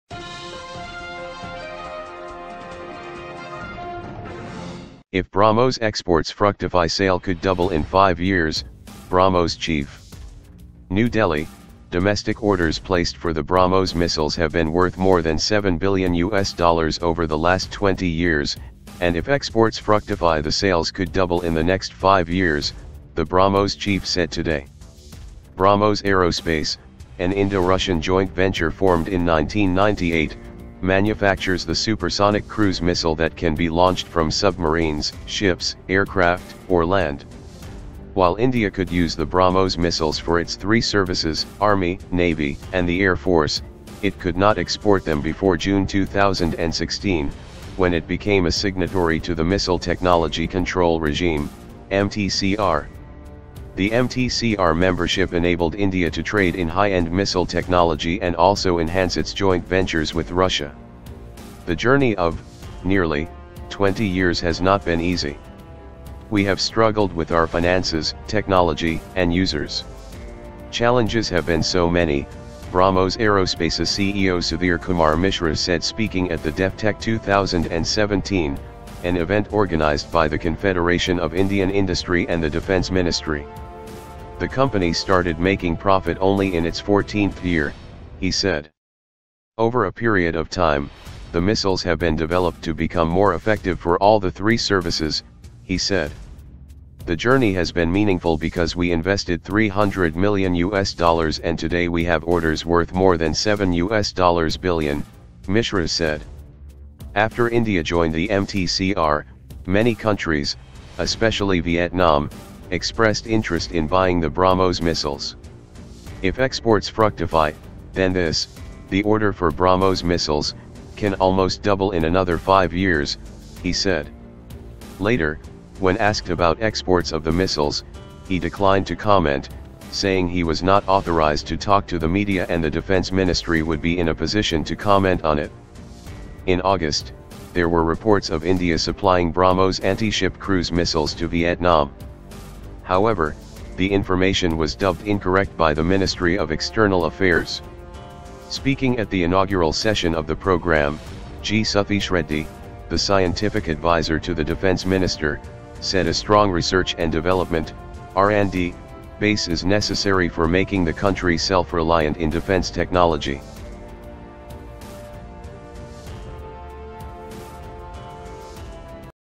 If BrahMos Exports Fructify Sale Could Double In 5 Years BrahMos Chief
NEW DELHI:  Domestic orders placed for the BrahMos missiles have been worth more than USD 7 billion over the last 20 years, and if exports fructify the sales could double in the next five years, the BrahMos chief said today.

BrahMos Aerospace, an Indo-Russian joint venture formed in 1998, manufactures the supersonic cruise missile that can be launched from submarines, ships, aircraft, or land.